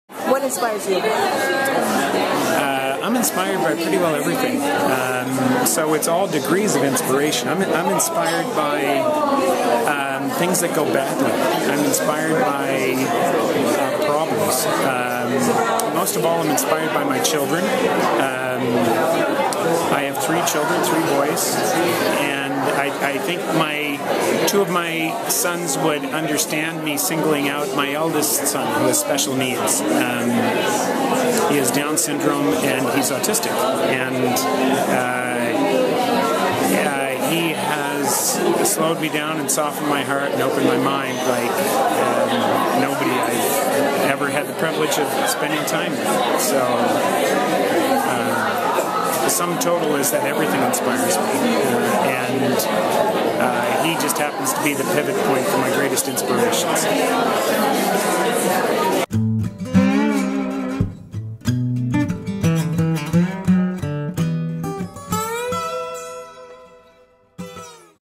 Danny Graham inspired
Danny Graham came to speak at ALIA summer institute. What inspires Danny?
All degrees of inspiration- things that go badly, problems, my children,
sum total everything inspires me, my special needs son happens to be the pivot point to my greatest inspiration.

clip + music by Cathy Grier NYCSubwayGirl